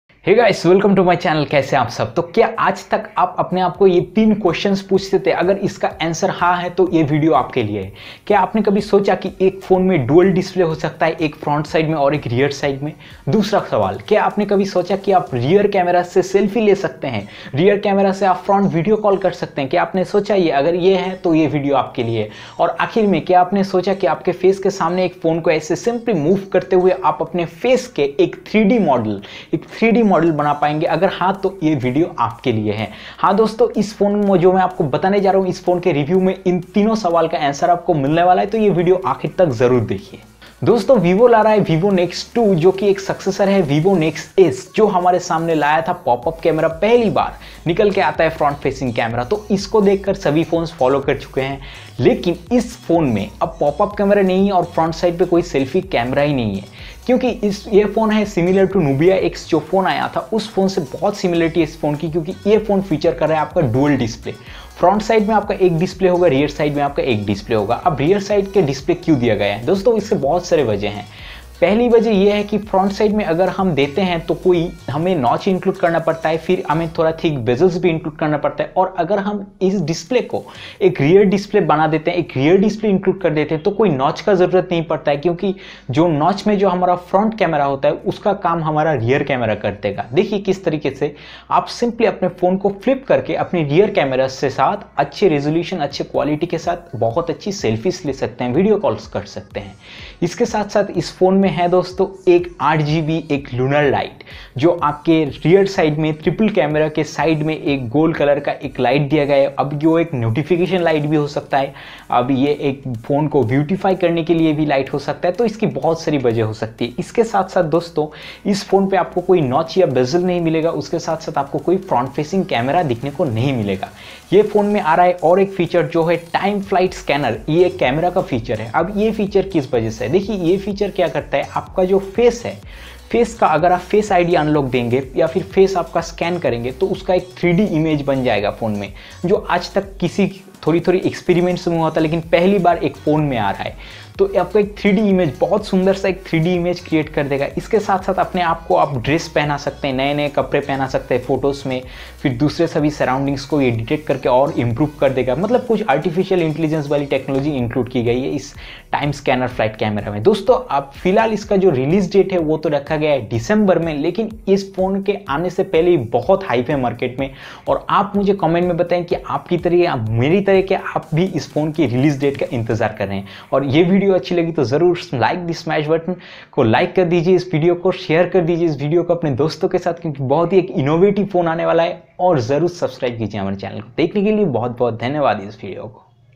Vivo nex 2 review | Vivo nex 2 price
Hey guys aagr aapne kabhi socha ki aagr ek phone me do screen hota to kya hot, aagr rear cameras k sath selfie le pate to kitna aacha hota, aagr aapne face ka ek 3d image bana paate to badiya hota to aapka intezaar yaha khatm hota h kyuki vivo launch kar raha h aapna next phone Nex 2.

Iss phone me aapko kuch aankhe features dekhne ko milenge jo market me oehli baar kisi smartphone me include kiye ja rahe h isisliye video ko aakhir tak dekhiye.